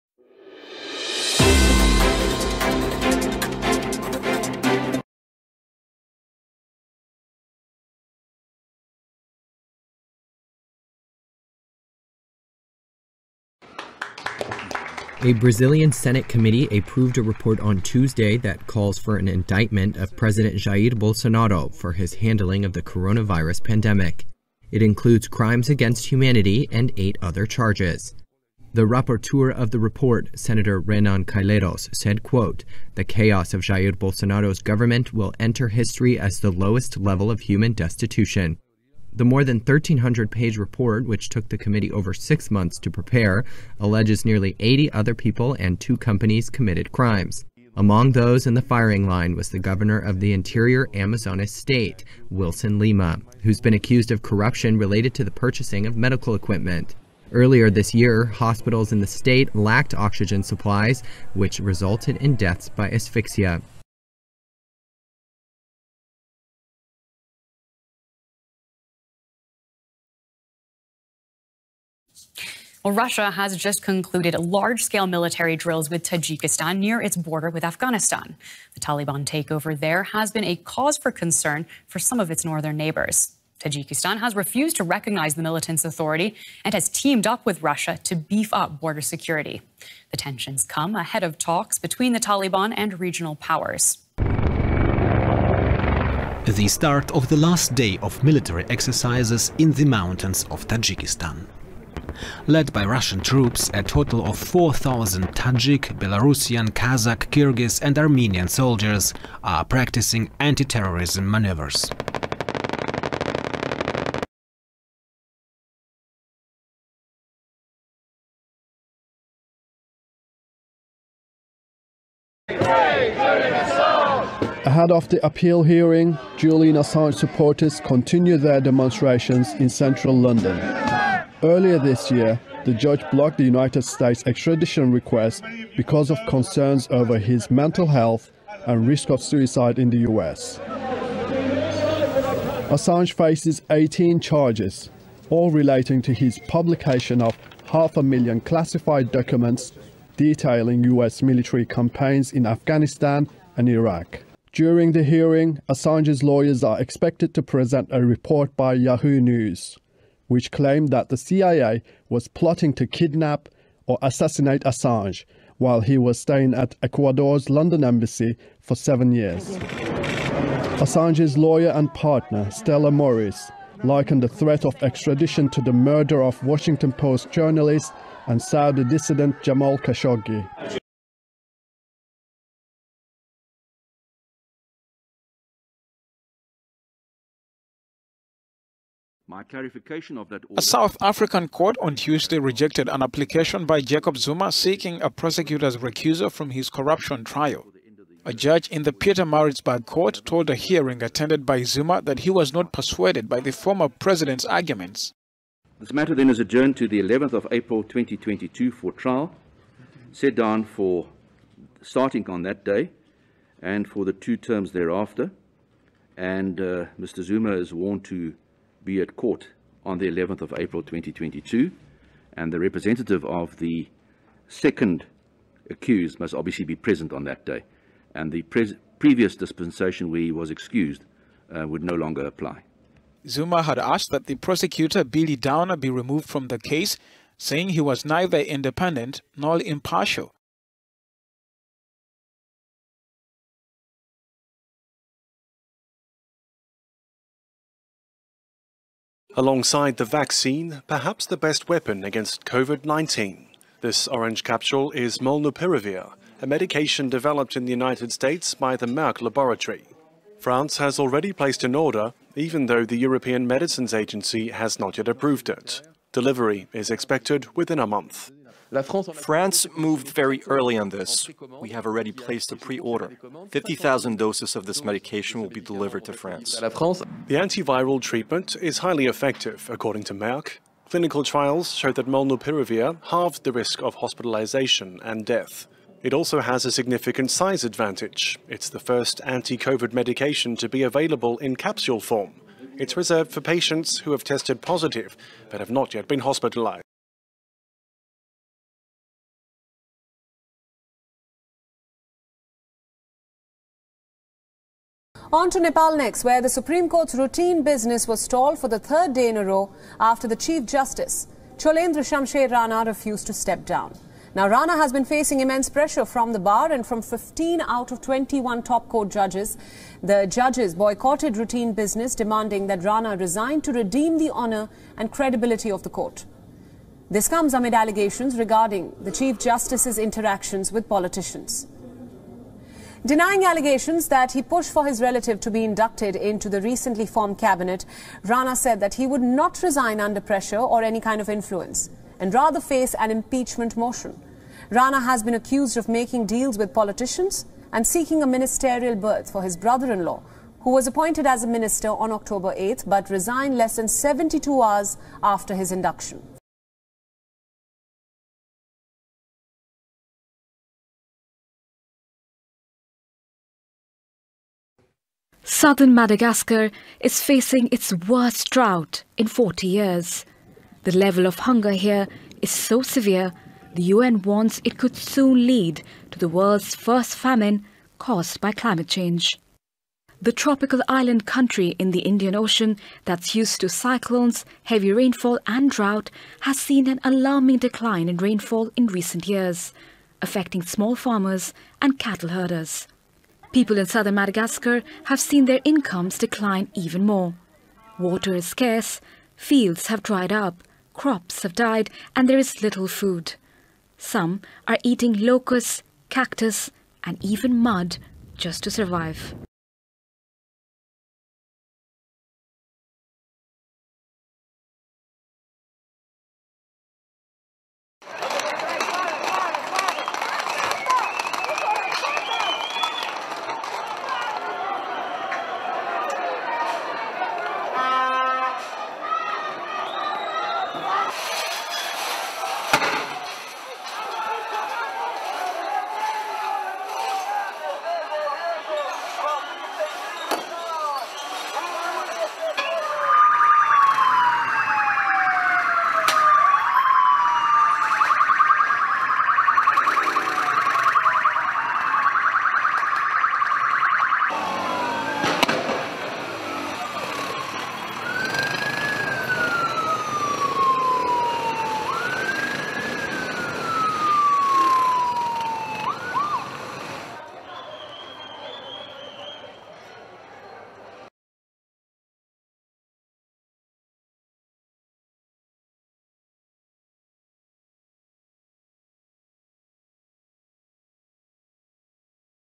PPN World News Headlines - 27 Oct 2021 • Russian Wargames • Bolsonaro vs Covid-19 • Julian Assange
Perpetual Pixel News
International world news headlines today.

► Sources:
00:06 - Senate committee calls for Bolsonaro charges
01:05 - Russia holds military drills on the Afghanistan border
01:55 - Julian Assange fights US efforts to extradite him
03:02 - Zuma loses plea to remove prosecutor
04:02 - France orders 50,000 doses of Merck's Covid-19 antiviral pill
04:58 - Chief Justice Rana refuses to step down
05:57 - More than one million edged towards famine
06:51 - Riot police clash with protesters blocking off roads